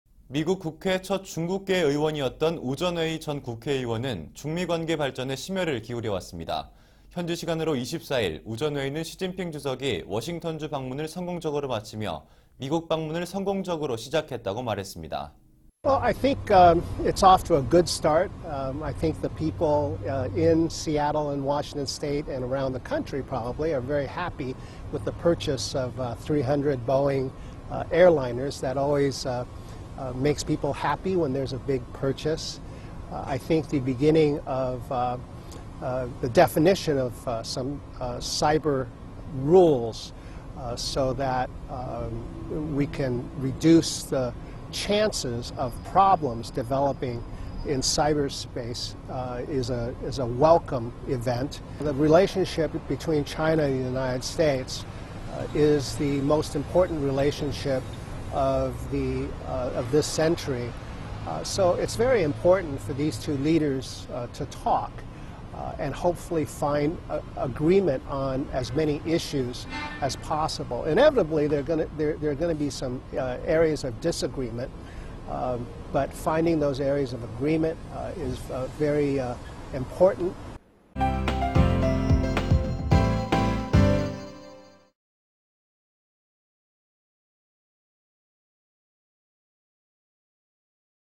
미국 전 국회의원, "시 주석 방美 양호한 시작"
미국 국회 첫 중국계 의원이었던 우전웨이 전 국회의원은 중미관계 발전에 심혈을 기울여 왔습니다.

현지시간으로 24일 우전웨이는 시진핑 주석이 워싱턴주 방문을 성공적으로 마치며 미국 방문을 성공적으로 시작했다고 말했습니다.